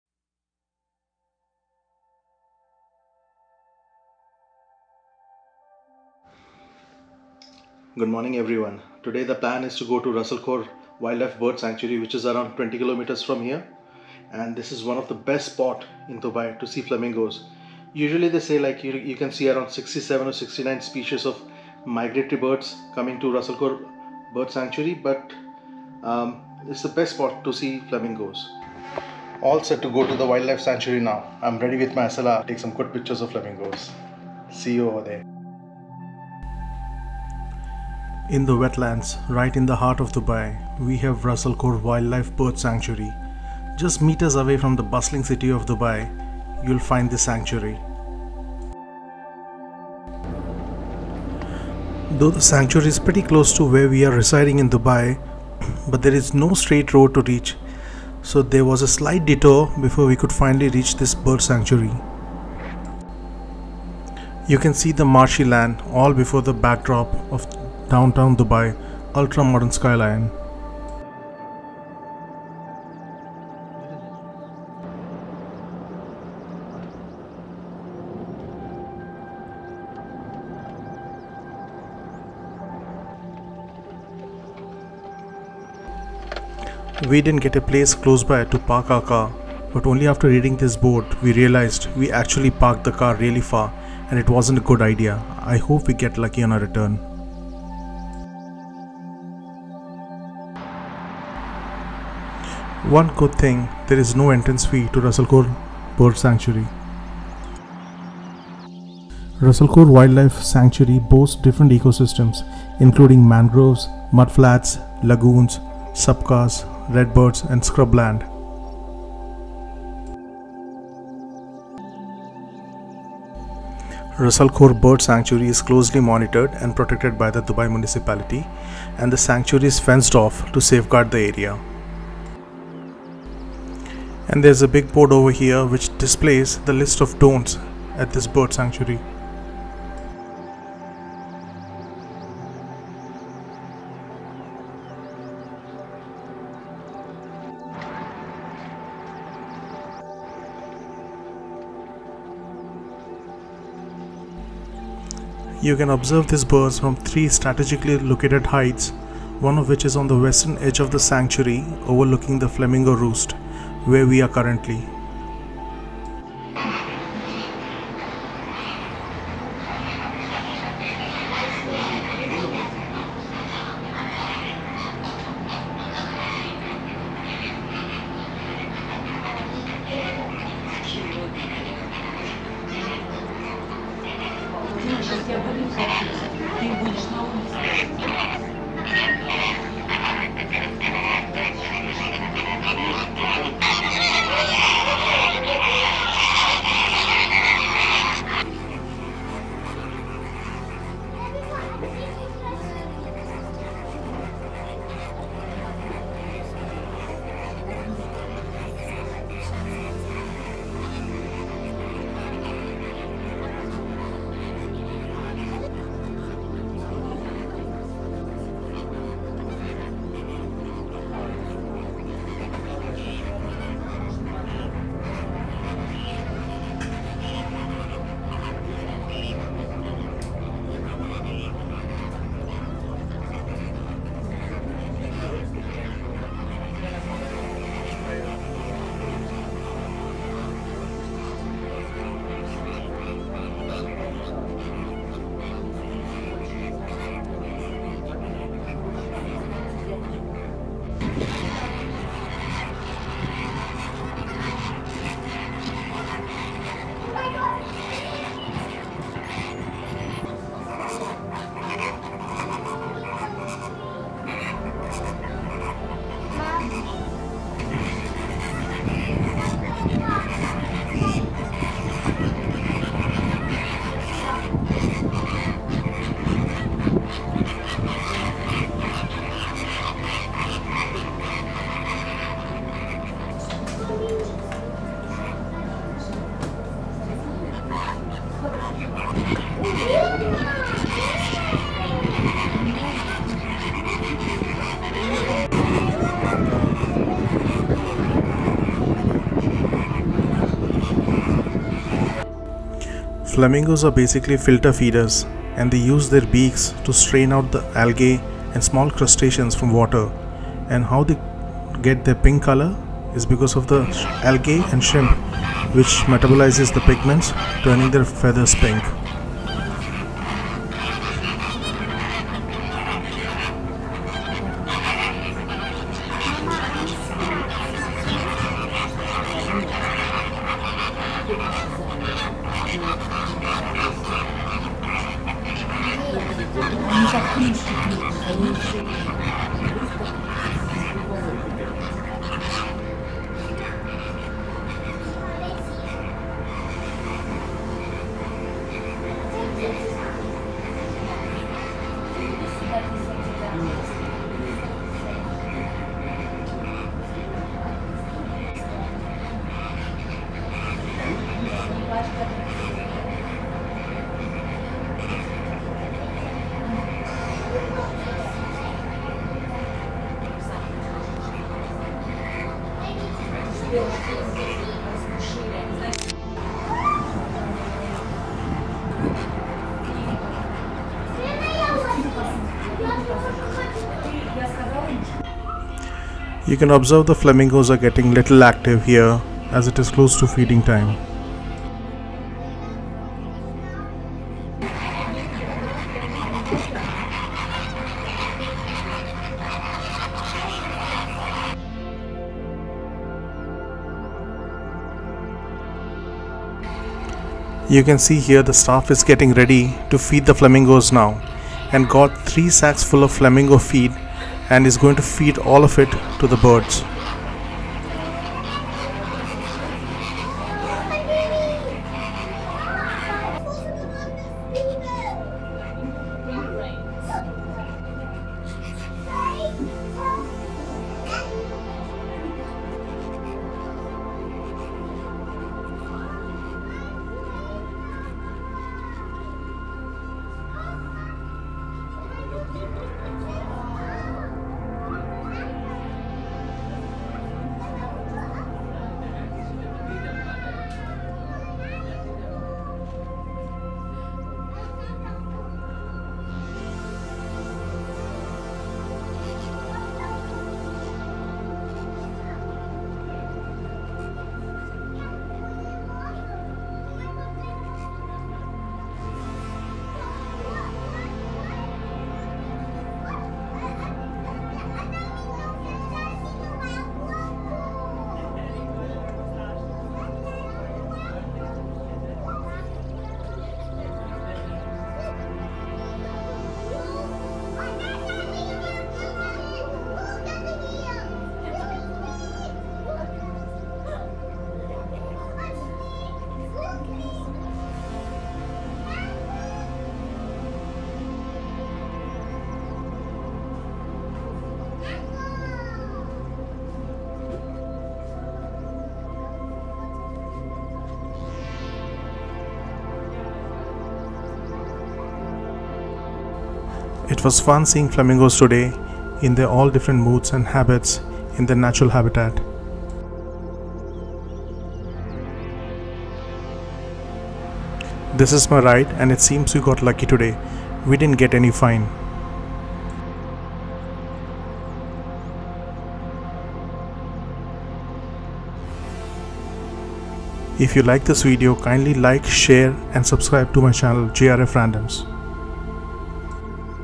Ras Al Khor Wildlife Bird Sanctuary | Flamingos | Mangroves | Raw Video | Dubai
In the wetlands, right in the heart of Dubai, we have Ras Al Khor Wildlife Bird Sanctuary, just meters away from the bustling city of Dubai, you’ll find this Sanctuary. You can see the marshy land here, all before the backdrop of Downtown Dubai ultra-modern skyline. One good thing, there is no entrance fee to Ras Al Khor Bird Sanctuary. Ras Al Khor Wildlife Sanctuary boasts different ecosystems including mangroves, mudflats, lagoons, sabkhas, red beds and scrubland. Ras Al Khor is closely monitored and protected by the Dubai Municipality and the sanctuary is fenced off to safeguard the area. And there is a big board over here which displays the list of DONTS at this bird sanctuary. You can observe these birds from three strategically located hides, one of which is on the western edge of the sanctuary overlooking the flamingo roost, where we are currently. There are six different species of flamingos in the world and the ones you’ll see at Ras Al Khor wildlife sanctuary are Greater Flamingos, a species native to the UAE. Flamingos are basically filter feeders and they use their beaks to strain out algae and small crustaceans from water and “how they get their pink colour” – is because of the algae and shrimp which metabolizes the pigments, turning their feathers pink.